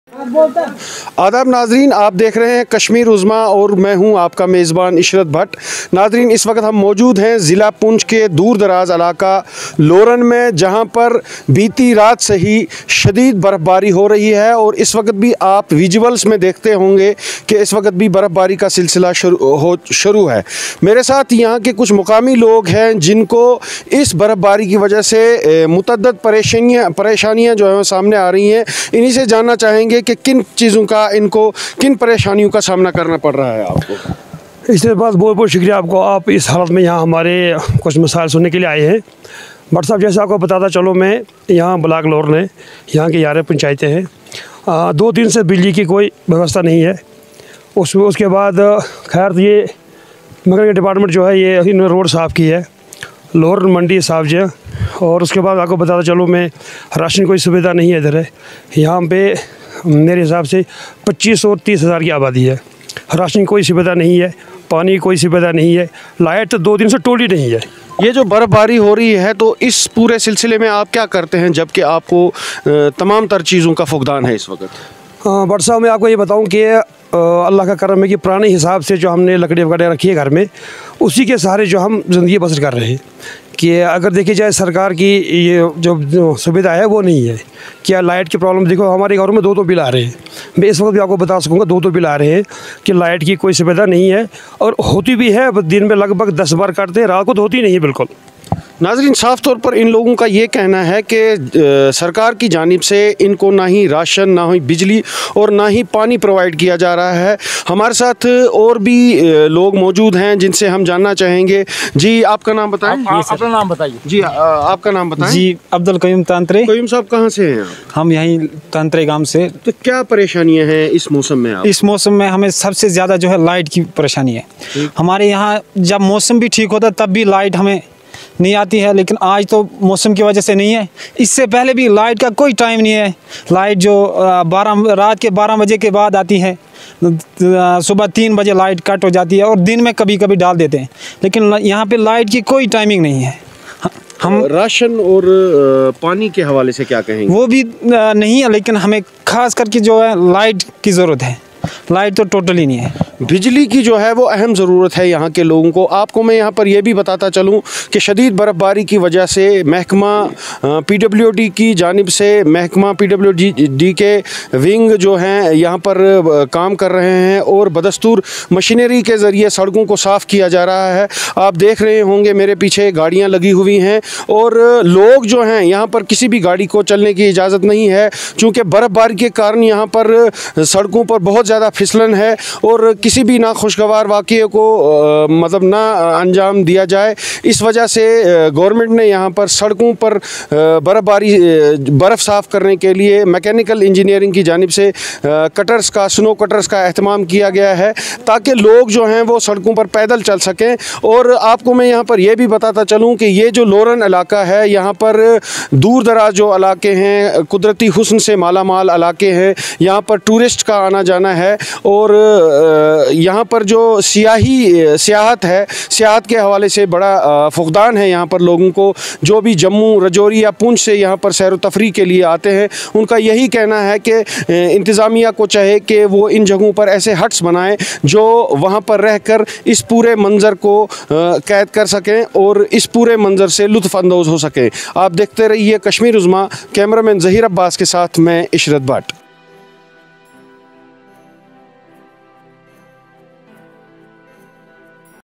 پونچھ کے پہاڑی علاقوں میں برف باری،معمولاتِ زندگی متاثر
ضلع پونچھ کے بالائی علاقوں میں بھاری برفباری کے بعد معمولاتِ زندگی متاثر ہو کر رہ گئے ہیں، علاقے میں بجلی نظام مکمل طور پر ناکارہ ہو چکا ہے۔ اسی دوران بالائی علاقوں میں برفانی تودے گرآنے کی وارننگ بھی جاری کی گئی ہے۔